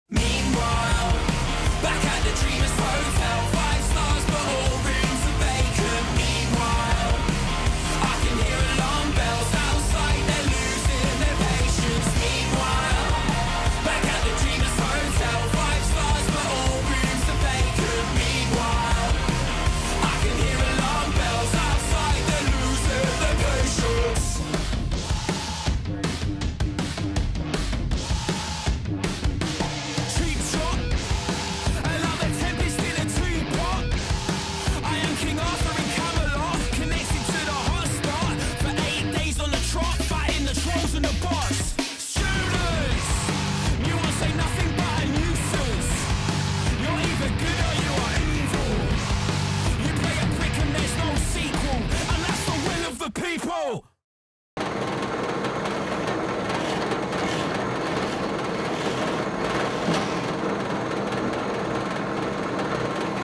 The Making of Rob Rolfe's White Sycamore Legend Series Kit
Enter Shikari's Rob wanted a one of a kind build. So we delivered!

Handpicked veneer and a high gloss lacquer is what makes this kit truly special!